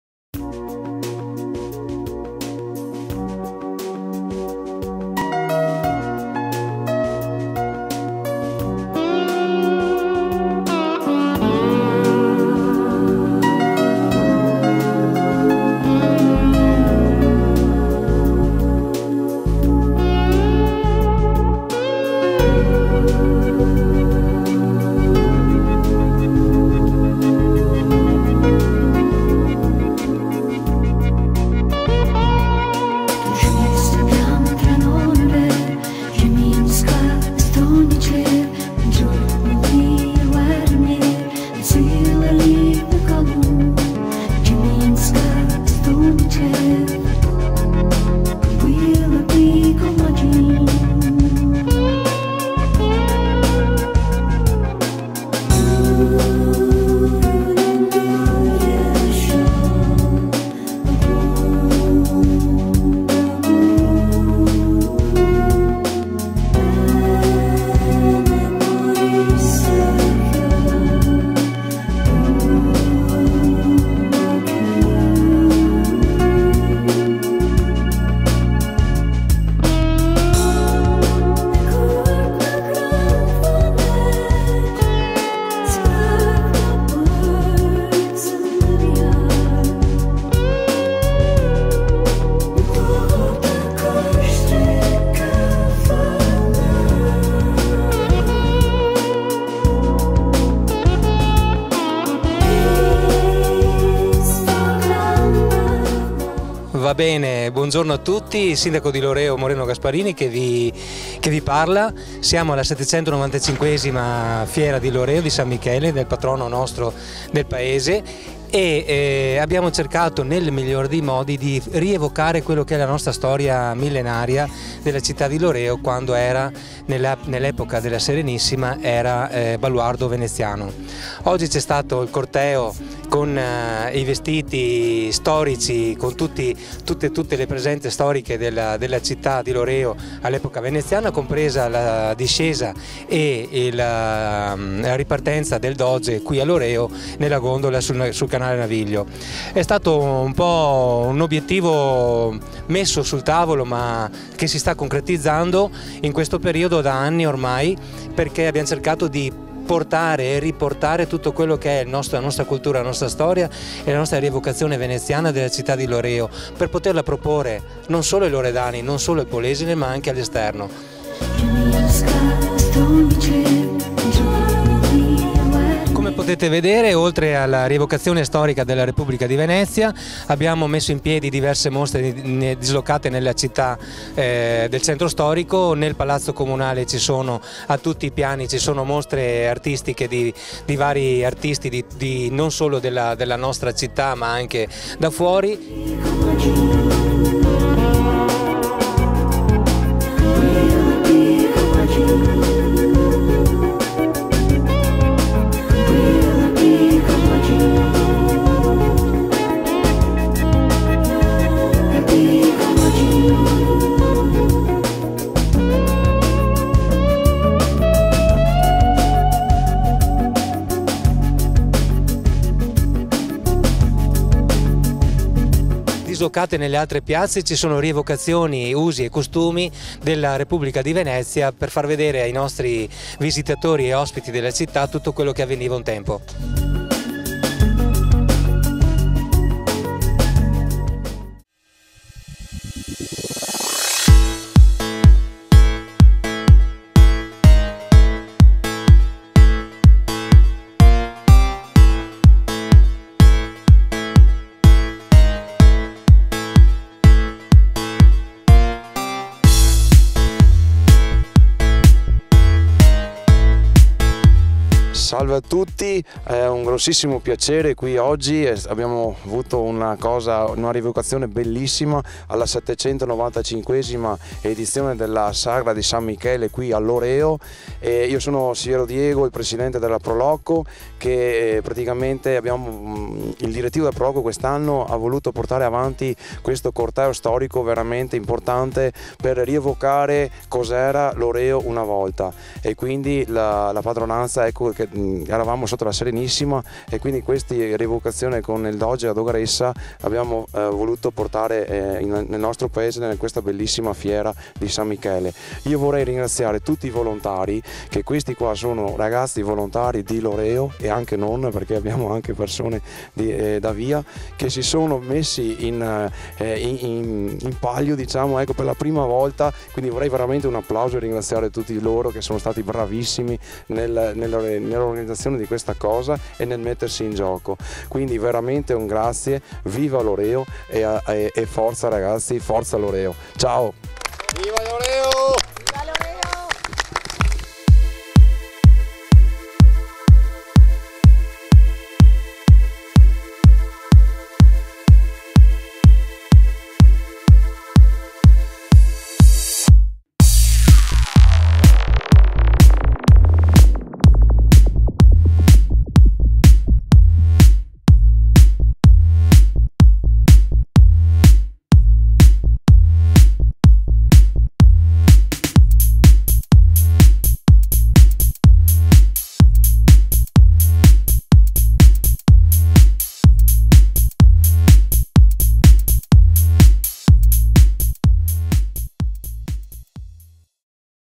795^ Antica Fiera di San Michele - Loreo ( RO )
Breve sintesi video della 795^ edizione della Antica Fiera di San Michele,svoltosi nella ridente cittadina di Loreo ( RO ).